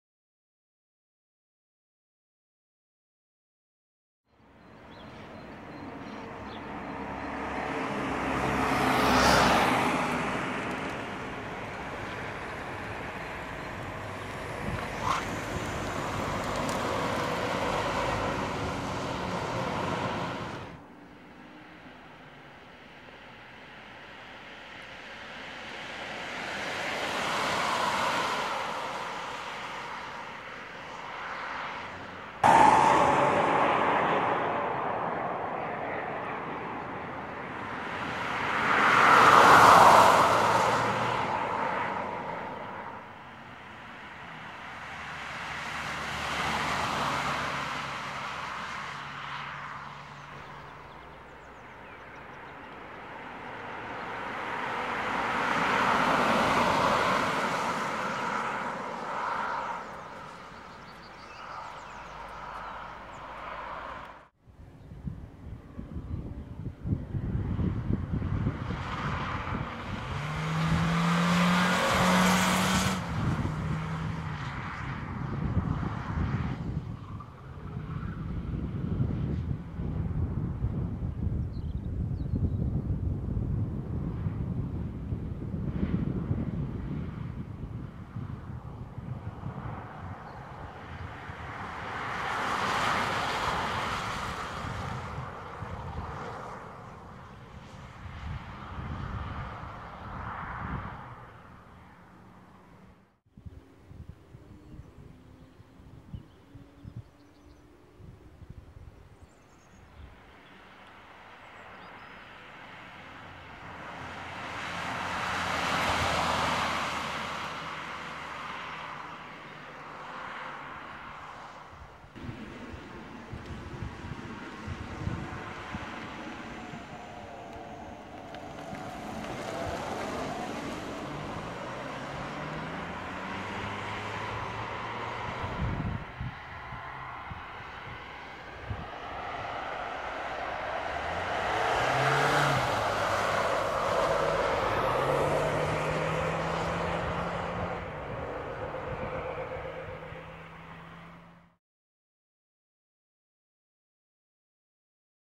Next-generation Honda Jazz / Fit spy video
The little hatchback wears a bulbous shape that doesn't match the sharper styling from the current crop of Honda models.